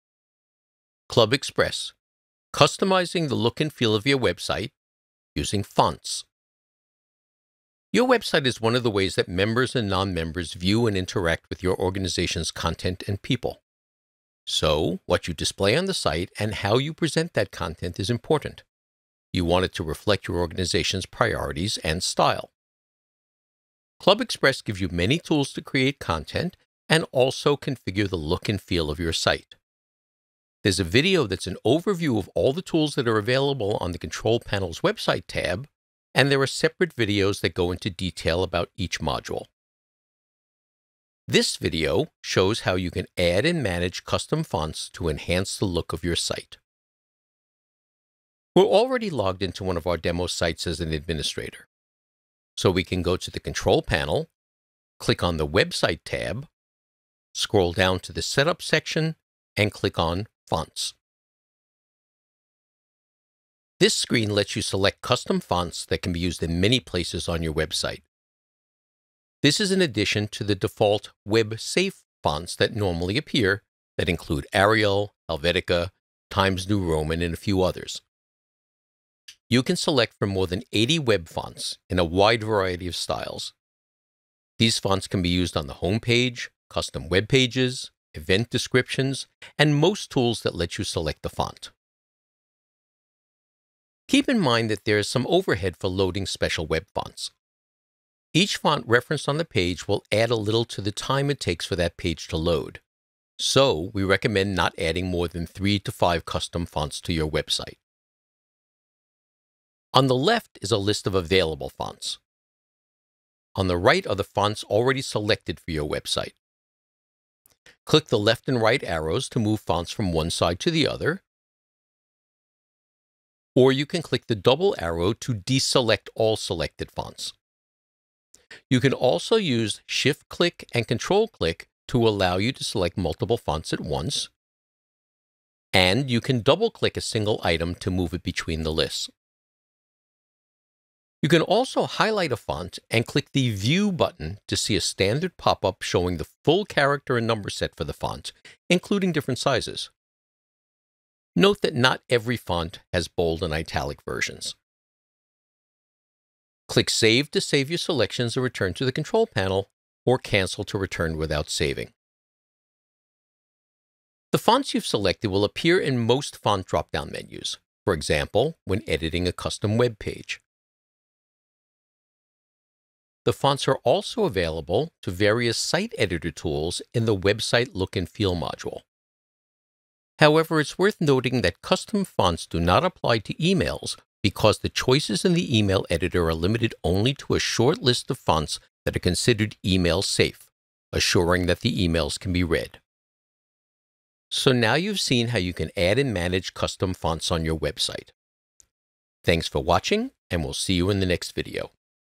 ClubExpress - 05b Customizing the Look and Feel of Your Website - Fonts
NOTE: We have updated our Look & Feel tools. To learn how to use the NEW Look & Feel tools, see our webinar and tutorial for the Template Builder.

In this video, learn how you can add and manage custom fonts to enhance the look of your site.